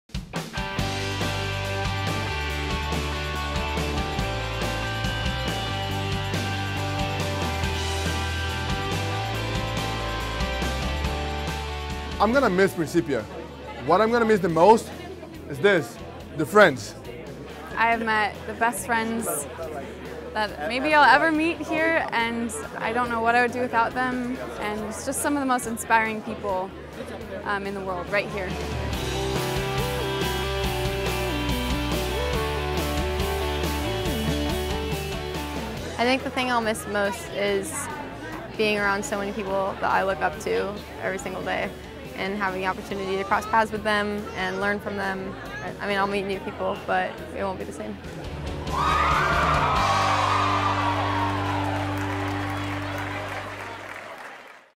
Commencement Highlights 2017
Enjoy some Commencement highlights and hear firsthand what graduates will miss about Principia.